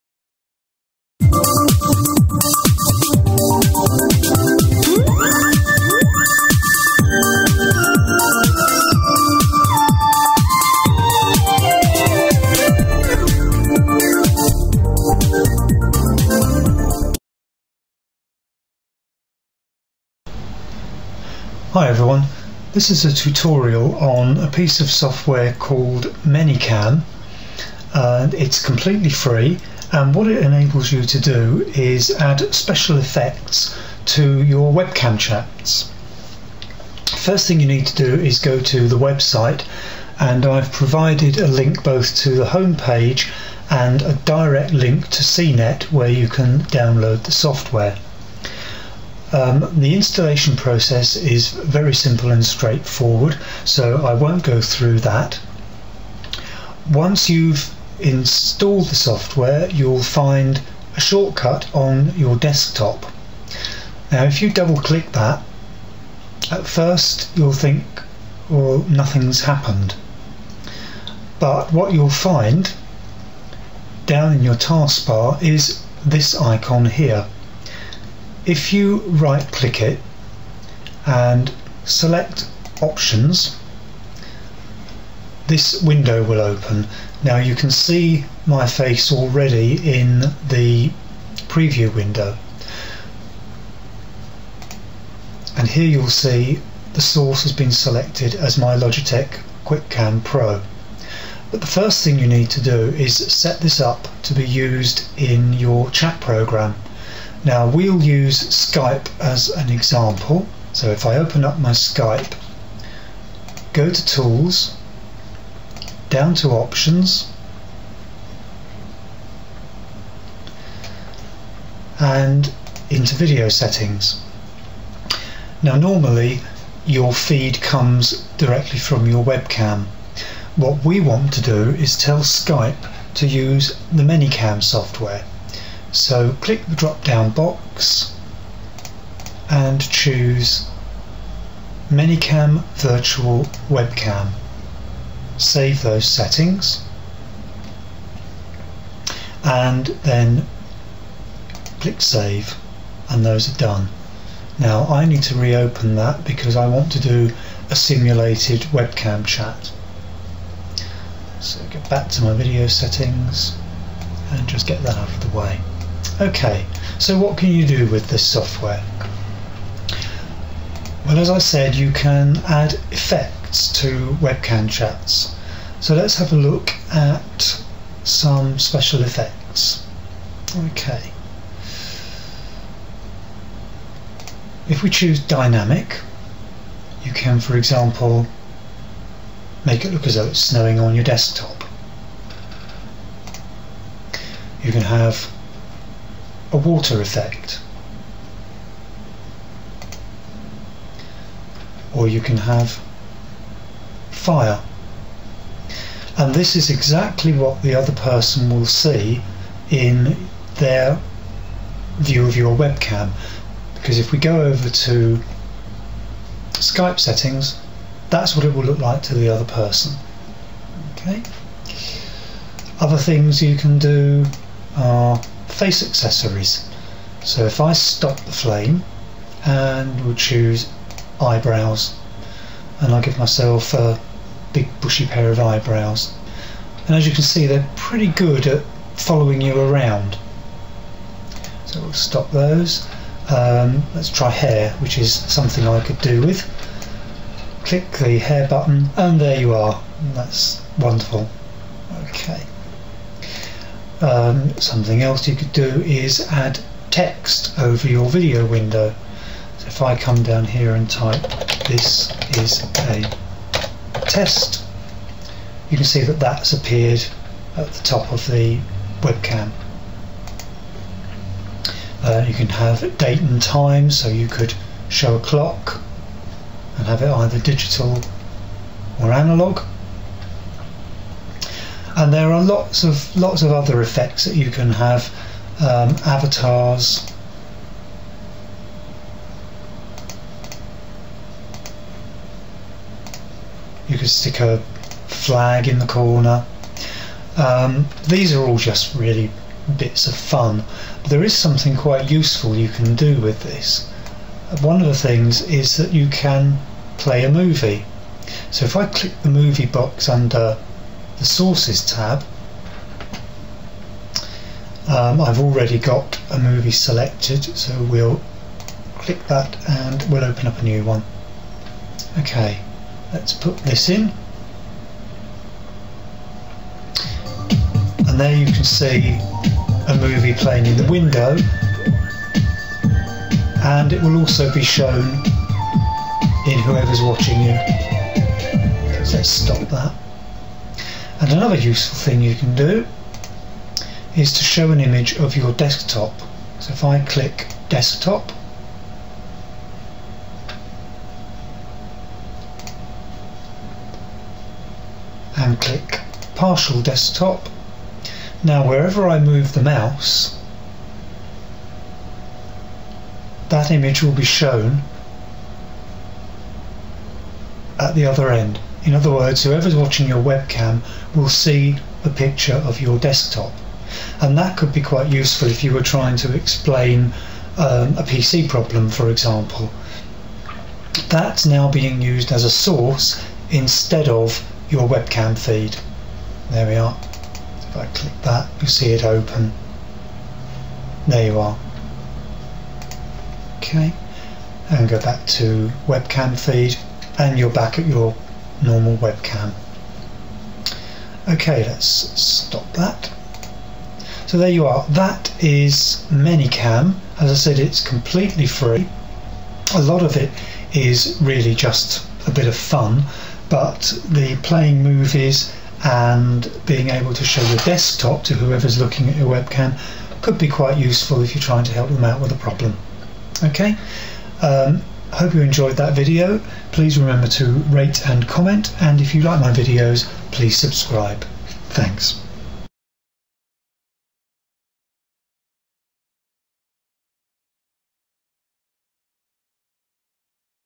Manycam - Add Special Effects to your Webcam Chats
This software allows you to add many different types of effect to a webcam chat, including playing a movie to your viewer and showing them your PC desktop live.